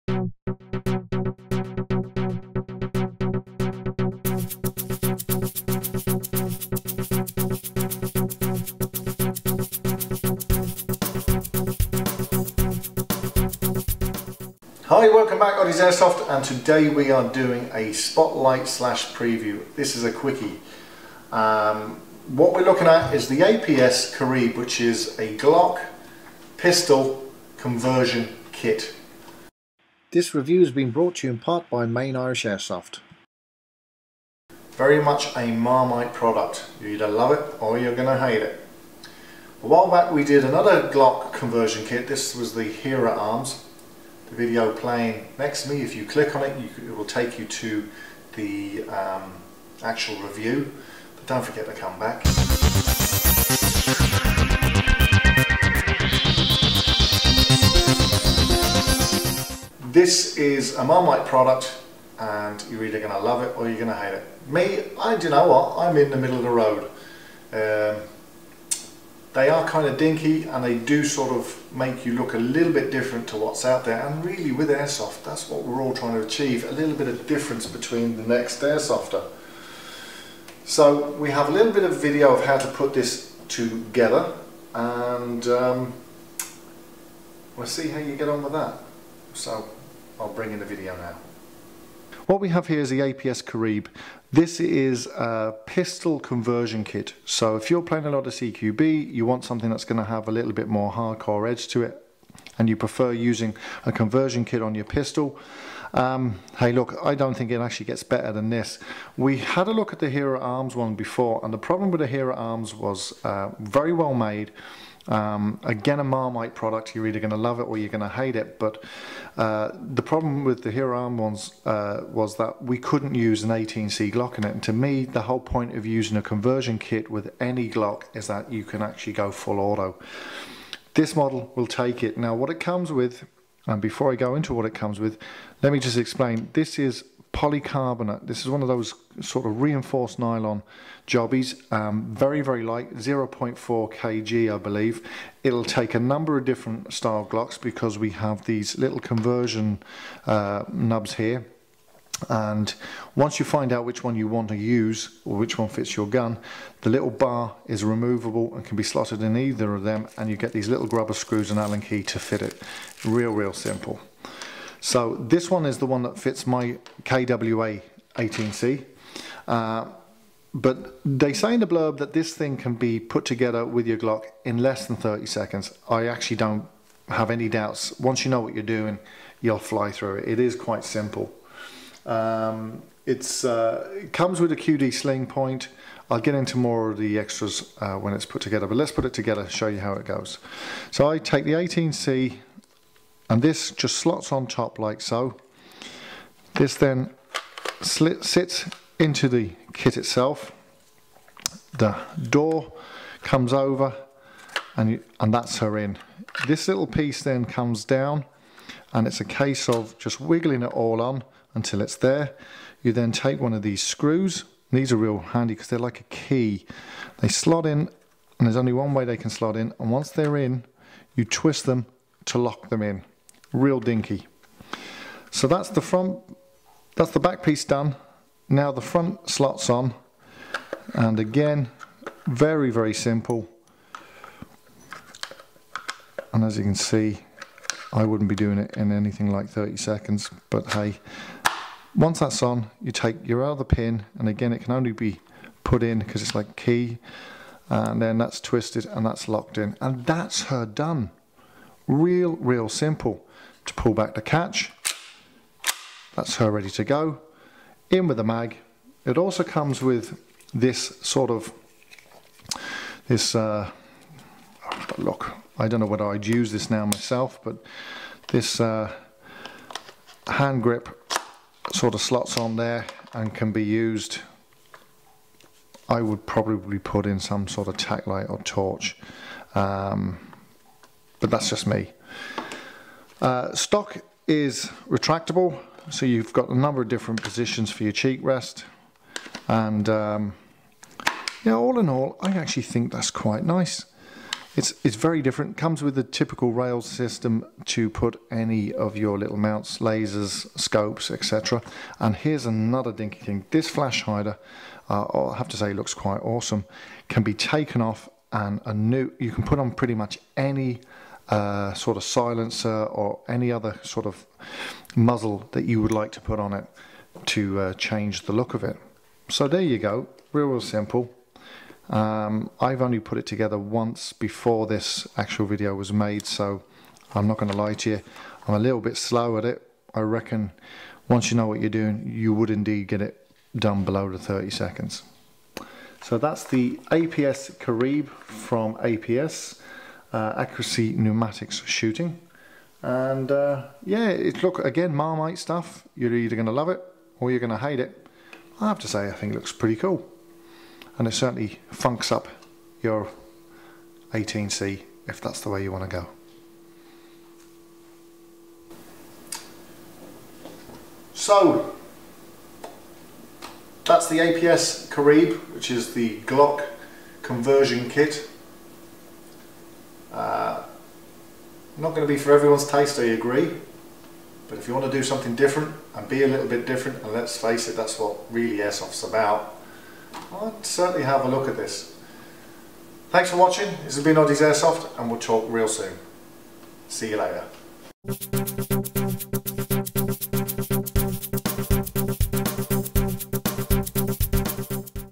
APS Caribe Glock Conversion Kit
I take a look at the new APS Caribe Glock Conversion Kit